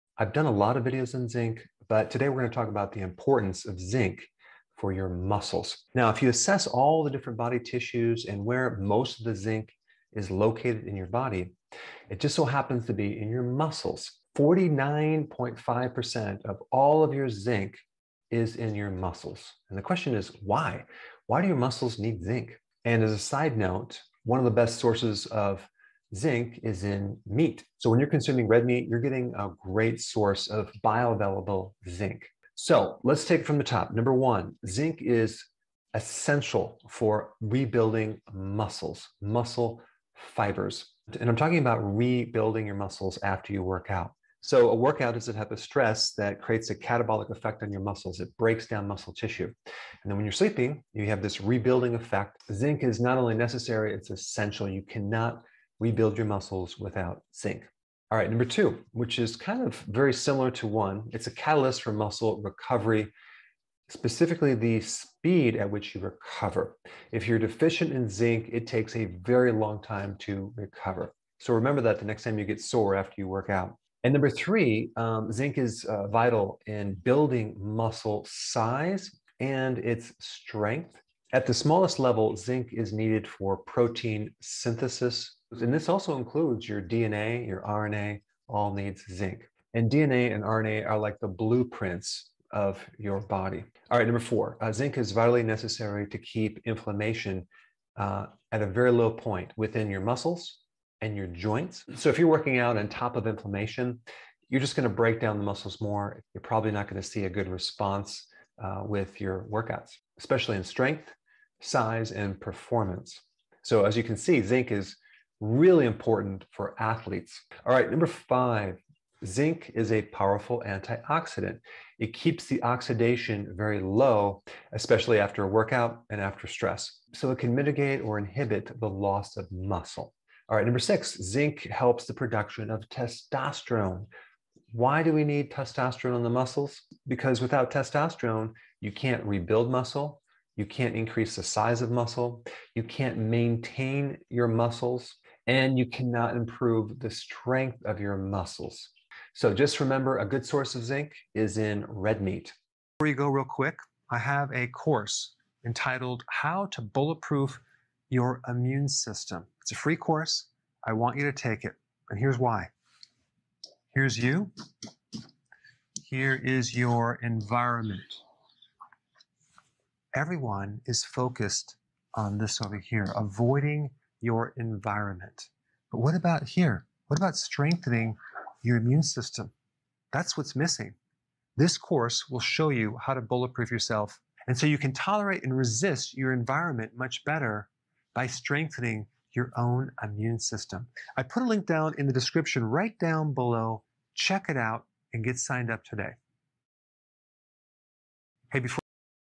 鋅對增強肌肉很重要,柏格醫生 Dr Berg
為什麼鋅對肌肉這麼重要？
時間標示：
0:00 簡介：肌肉與鋅
0:24 鋅的最佳來源
0:33 為什麼肌肉需要鋅？
2:33 增強免疫力！（免費課程只限美國）

柏格醫生dr berg談：鋅對肌肉的重要性。
體內49.5%鋅在肌肉中。

鋅的最佳來源是紅肉，吃紅肉會有大量生物可利用的鋅。
鋅對肌肉很重要：
1. 鋅對於運動後，重建肌肉纖維，很關鍵
2. 鋅是肌肉恢復的催化劑（特別是恢復速度）
3. 鋅在長肌肉、和力量，至關重要（組成蛋白質需要鋅）
4. 鋅是避免肌肉發炎、關節炎
5. 鋅在運動後，維持抗氧化（防止肌肉流失）
6. 鋅有助於產生睾丸激素（睾丸激素有助增長肌肉、加強肌肉力量）

艾瑞克柏格(Dr Eric Berg)醫生介紹：
56歲，脊椎醫生， 他是弗吉尼亞州、加利福尼亞州和路易斯安那州的3州脊椎科醫生，￼執照一直維持有效。他是全球知名的健康生酮飲食和斷食專家，也是《新體型指南》和KB Publishing出版書作者。